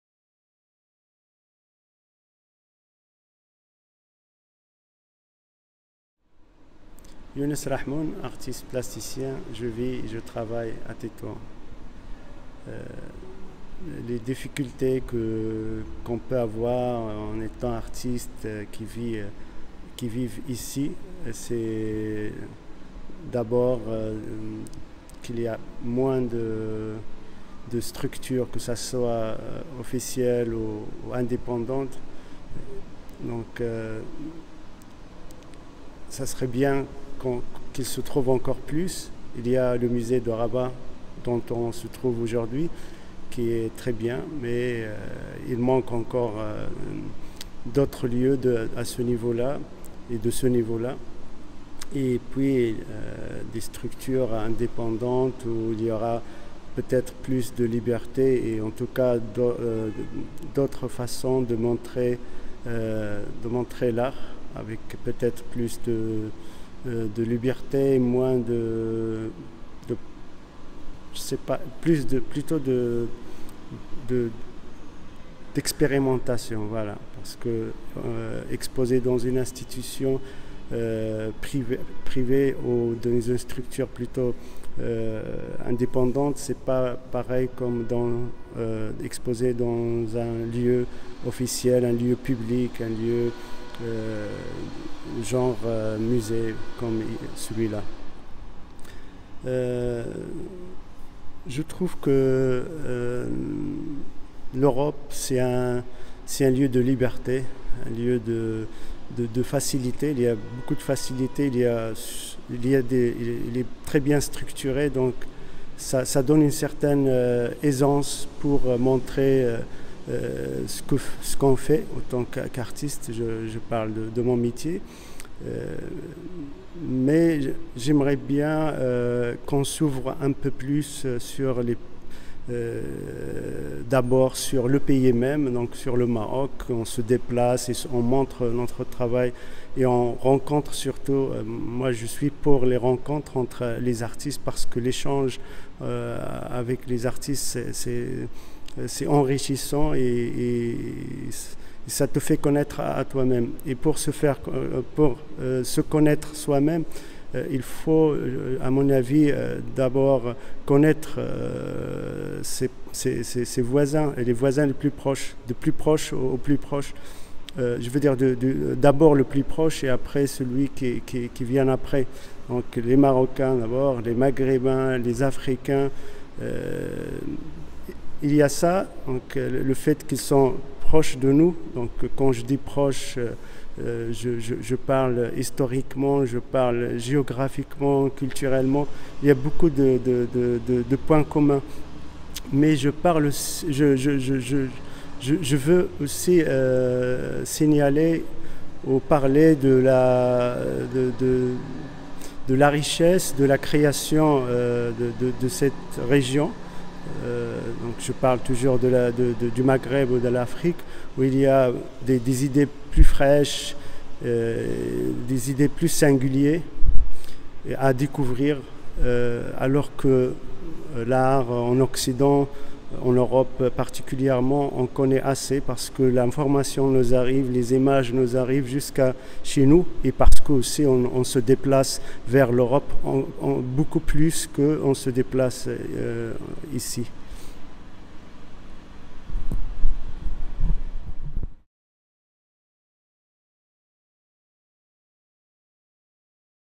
YOUNES RAHMOUN speaks of the state of art and culture in Morocco
An interview conducted by Marsad Drâa and commissioned by Arts Cabinet